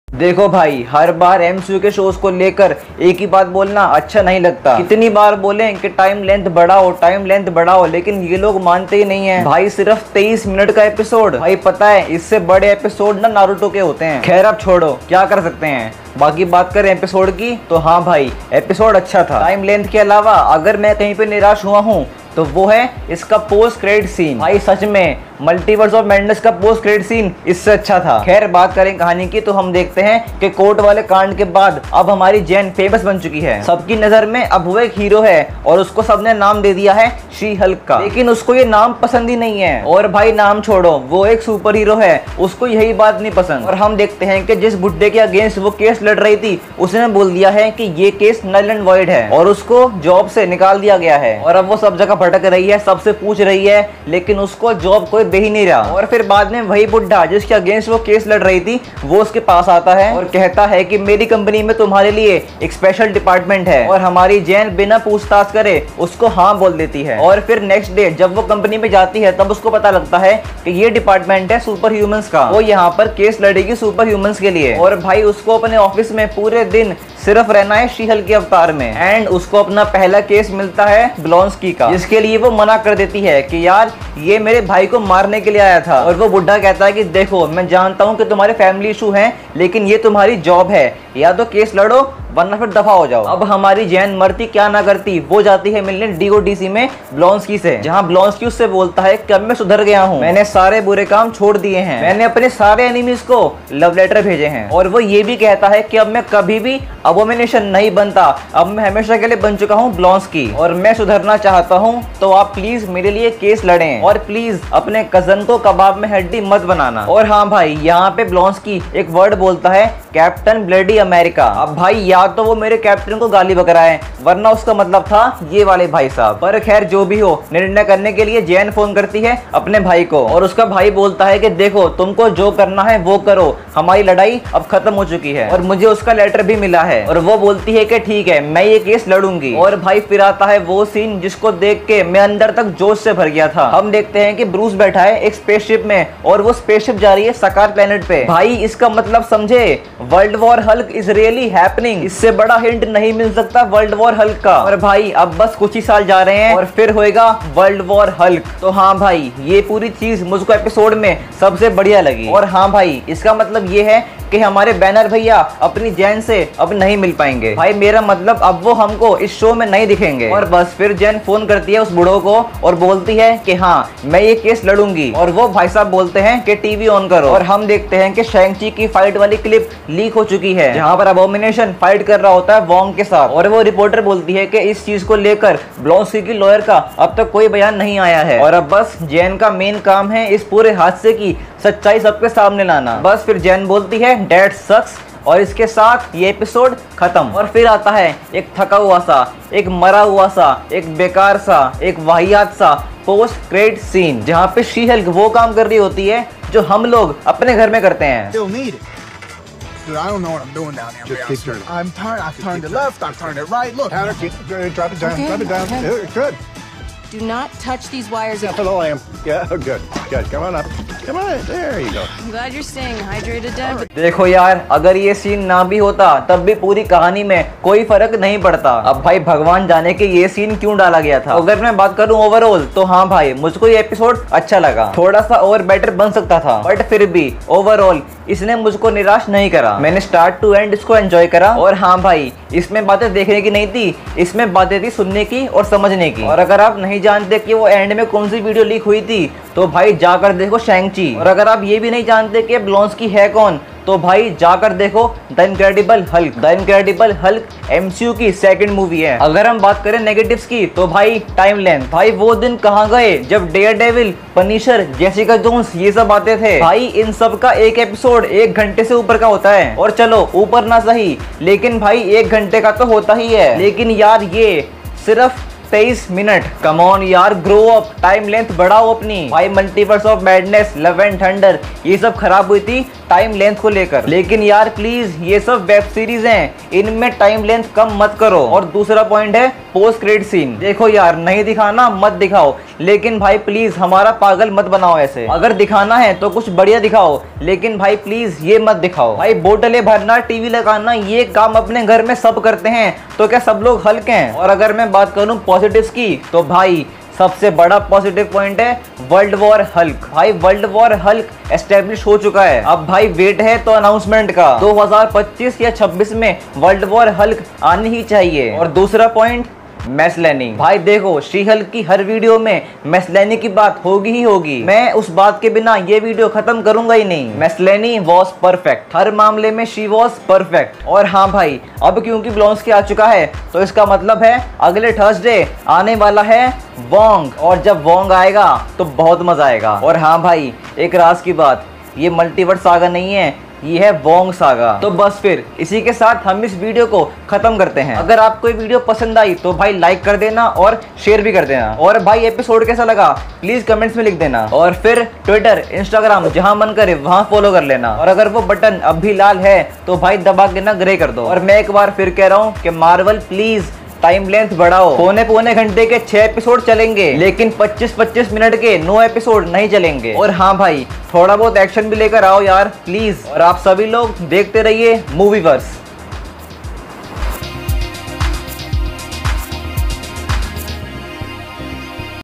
So fasten your seat belts: She Hulk Ep.2
In this video I've done a small review of She Hulk Ep.2 in hindi. I've explained the story and have given my opinion on this episode. The show features Tatiana Maslany as Jennifer Walters aka She Hulk, Mark Ruffalo as Bruce Banner aka Hulk.